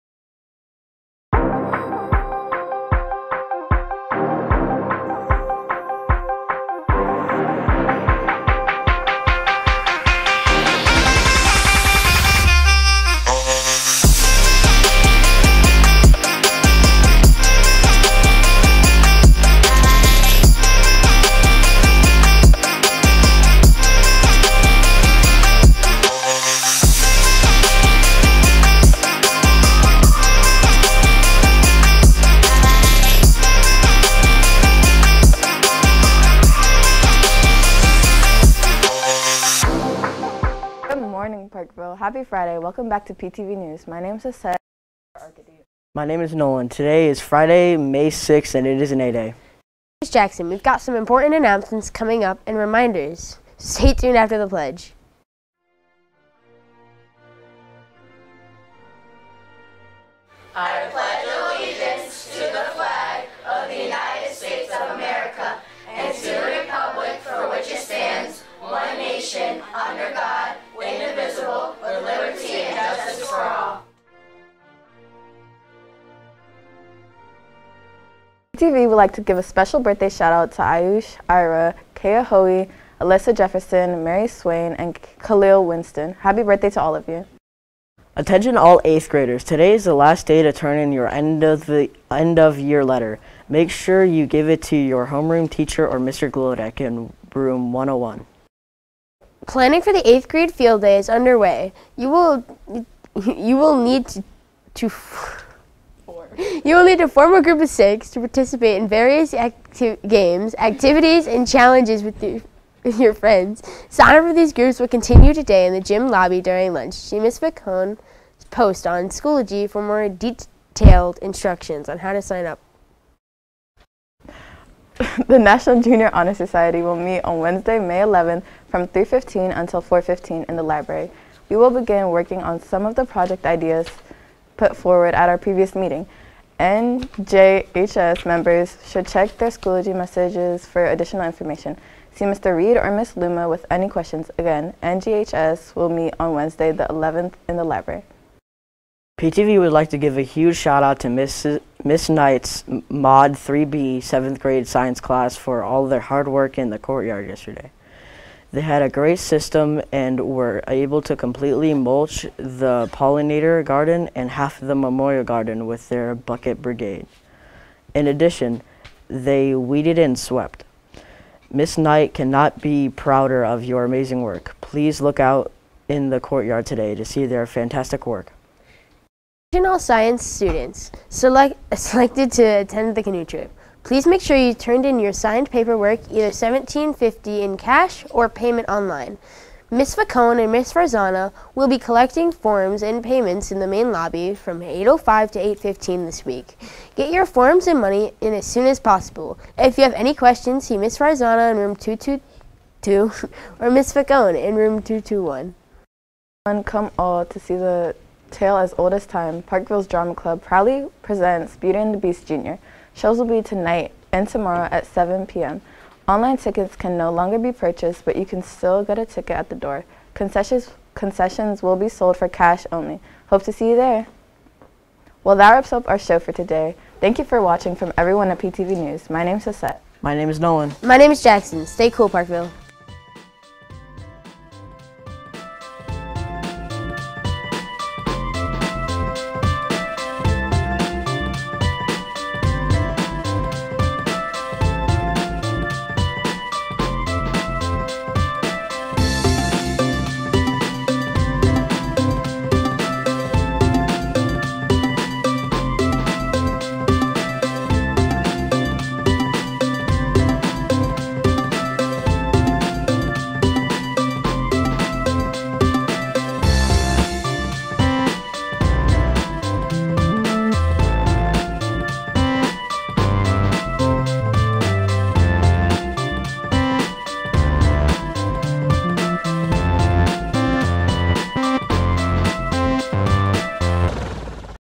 PTV News -- May 6, 2022
PTV News from May 6, 2022

PTV News is a daily show streamed live every school day at Parkville Middle School.

There are no copyright infringements intended for the use of the song(s) or picture(s) within this video.